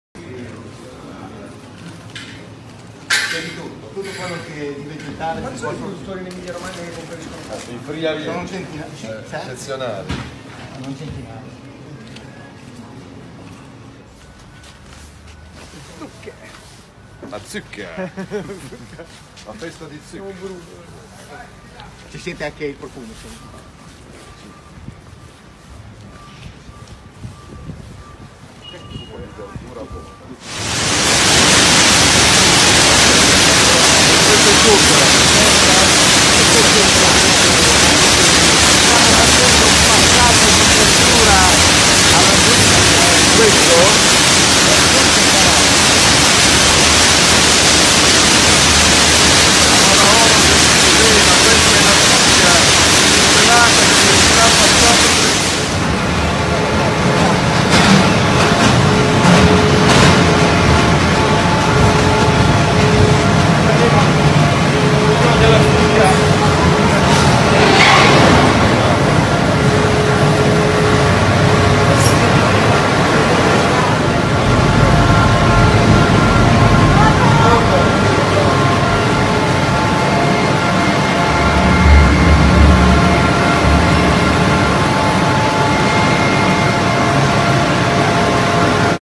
MATTEO SALVINI visita l’azienda di prodotti surgelati Orogel (11.11.2019)
Qui Cesena visitando l’azienda di prodotti surgelati Orogel, uno splendido esempio di cooperativa bella, sana, che reinveste gli utili, fa lavorare duemila agricoltori italiani e produce prodotti italiani.
Lunga vita alle nostre eccellenze, che vanno tutelate e valorizzate in Europa e nel mondo.